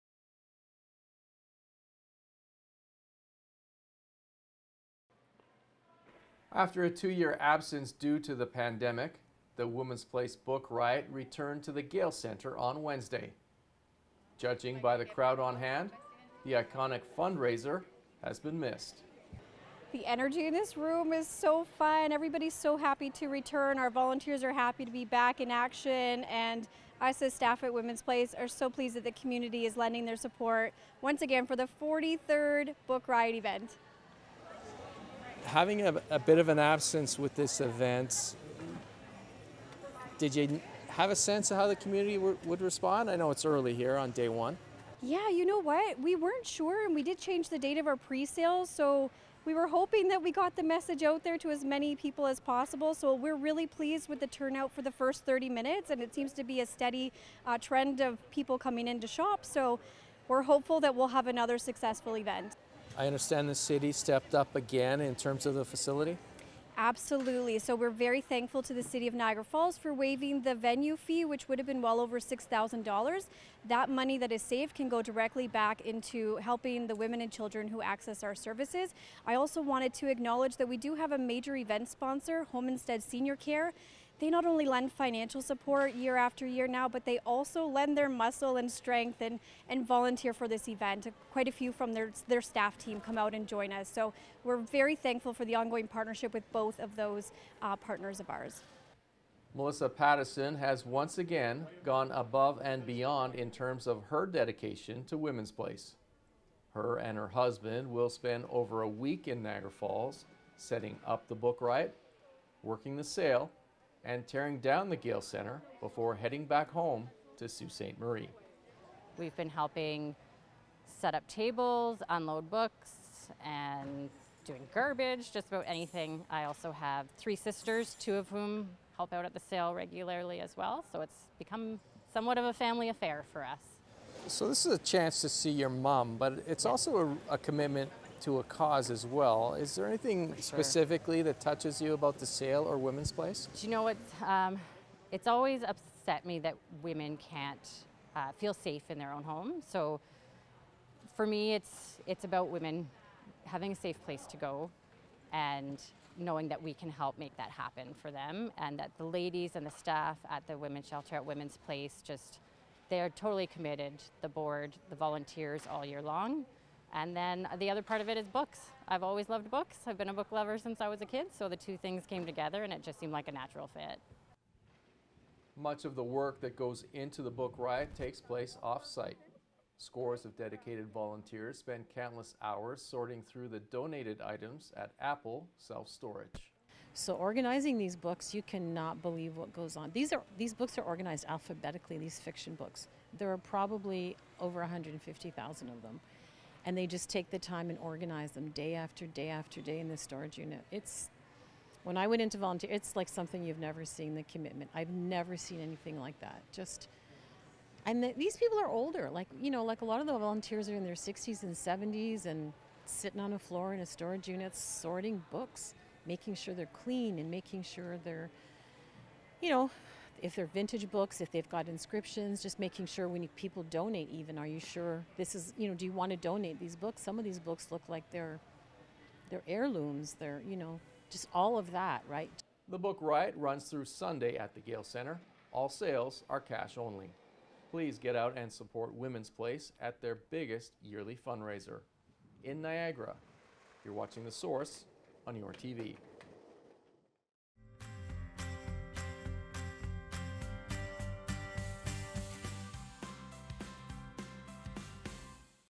The Book Riot Returns
Women's Place of South Niagara relaunched their popular Book Riot at the Gale Centre in Niagara Falls.